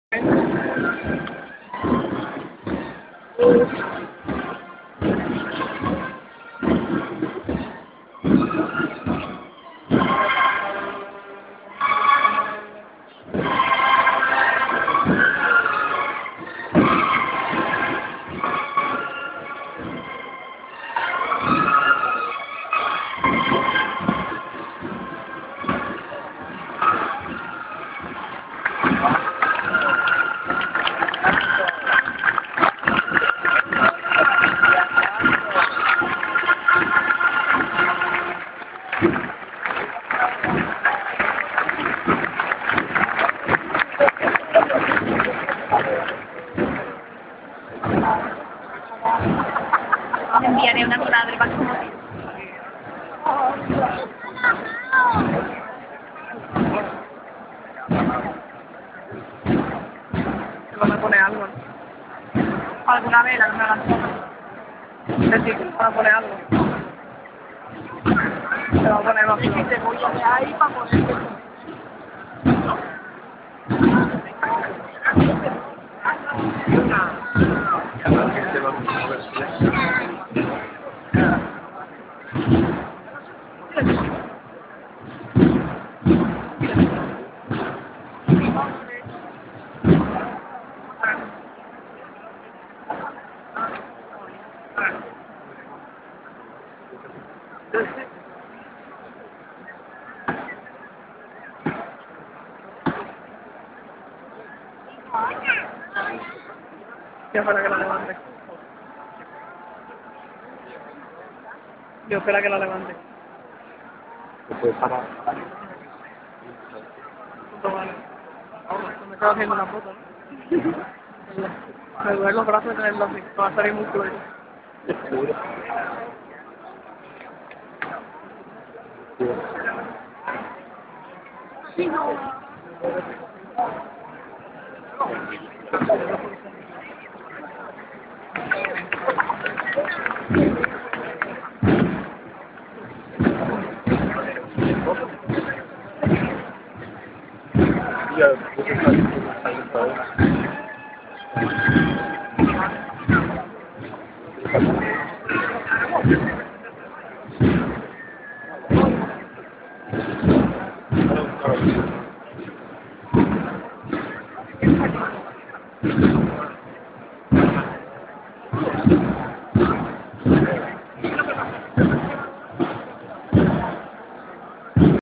Semana Santa de Sevilla 2013 Cristo de las Penas(La Estrella)
Vídeo de la hermandad de la Estrella grabado el Domingo de Ramos.